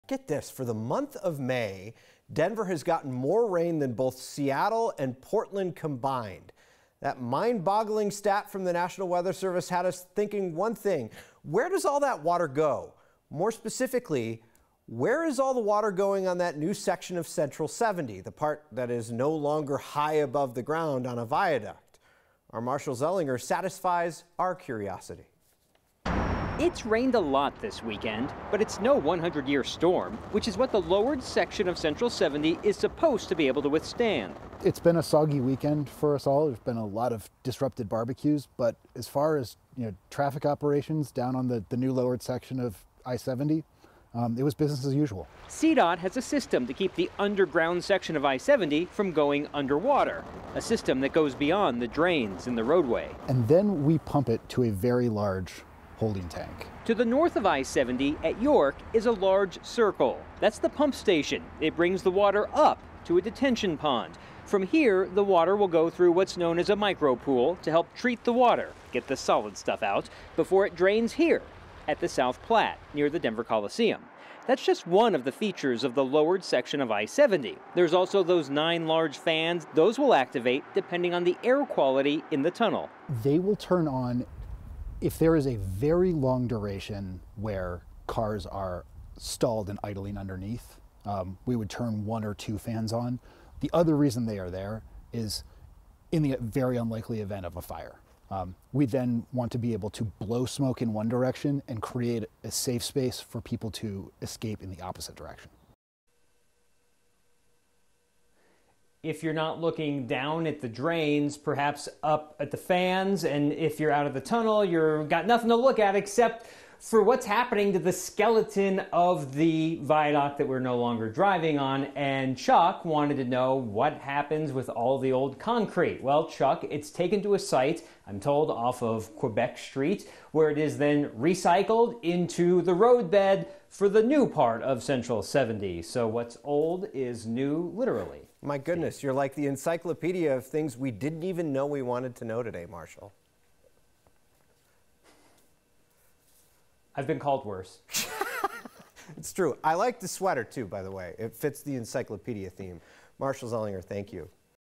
How does water drain from I-70's new underground tunnel?
This was a rainy May for Denver, which got us wondering: How does that work in Denver's new section of I-70?